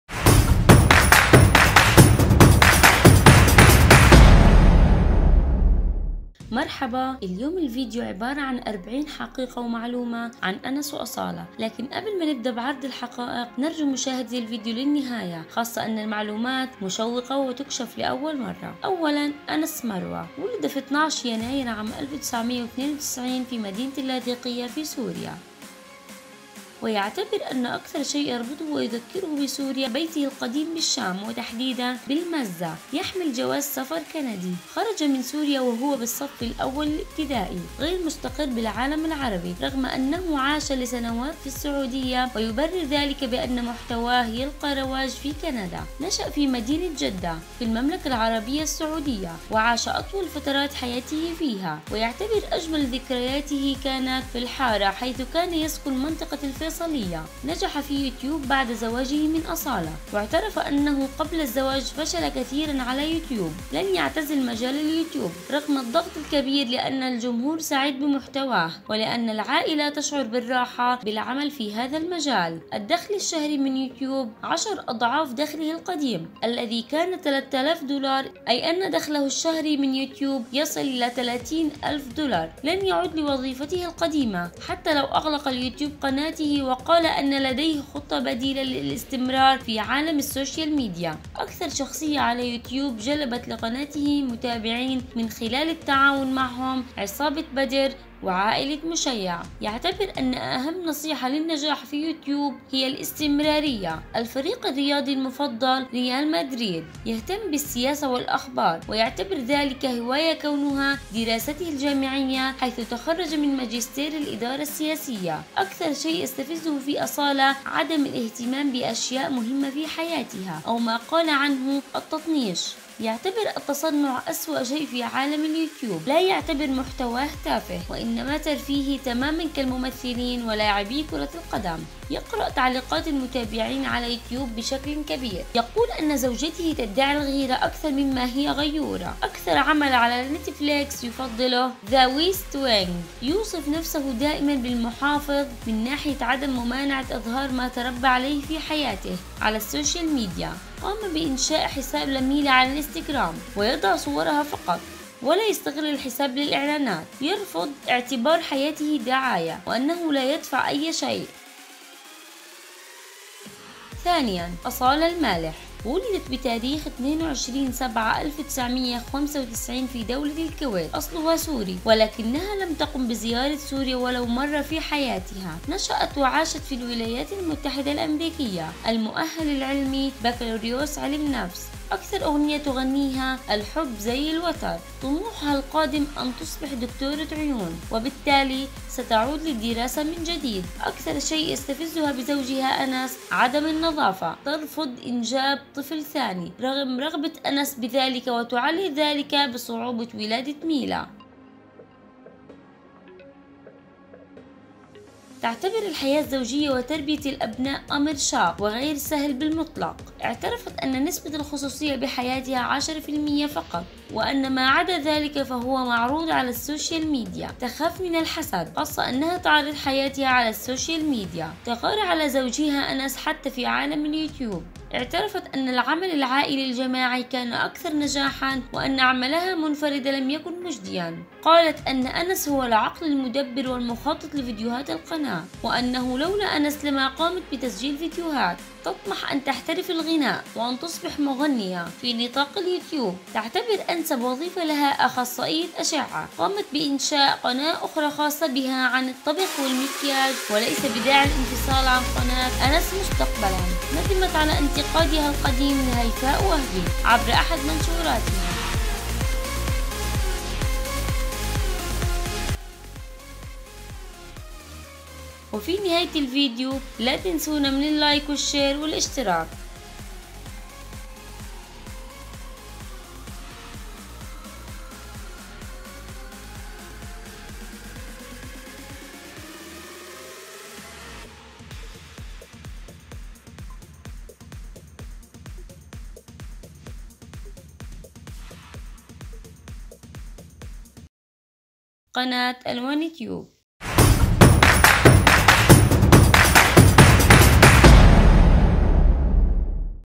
أرباح أنس وأصالة 30,000 دولار في الشهر😱 معقول حقيقي!؟ فيديو صادم 😲
الرقم الحقيقي لأرباح أنس وأصالة من يوتيوب هل فعلاً 30000 دولار، شاهد الفيديو لتعرف الحقيقة فالفيديو يتضمن حقائق ومعلومات وأسرار جديدة وحديثة عن اليويتوبرزأنس مروة وأصالة المالح وابنتهم ميلا
 انس و اصاله ، ارباح انس واصالة ومعلومات وحقائق وأسرار وخفايا انس واصالة 2020، كم عمر انس واصالة اصالة وانس من وين ؟